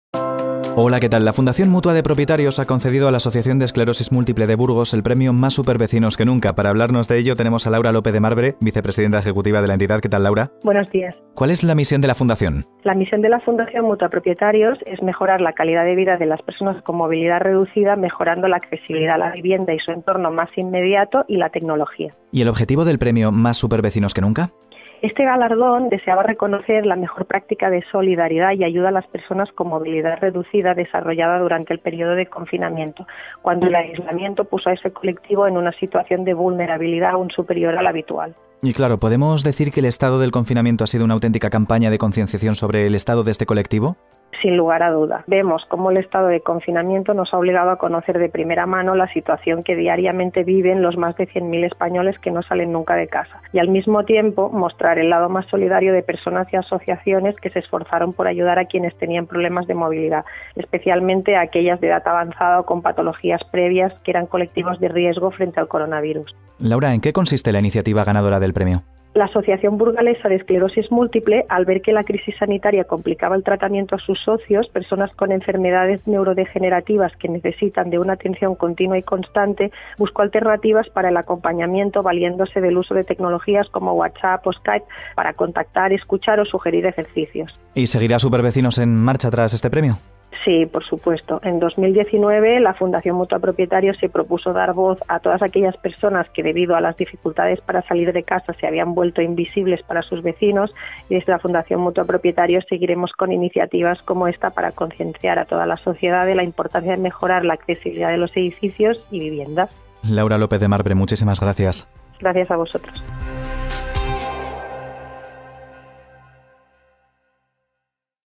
Entrevista a Laura López Demarbre en Cadena SER
Laura López Demarbre, Vicepresidenta Ejecutiva de la Fundación Mutua de Propietarios, habla en Cadena SER sobre la iniciativa Supervecinos.

"El estado de confinamiento nos ha obligado a conocer de primera mano la situación que diariamente viven los más de 100.000 españoles que nunca salen de casa"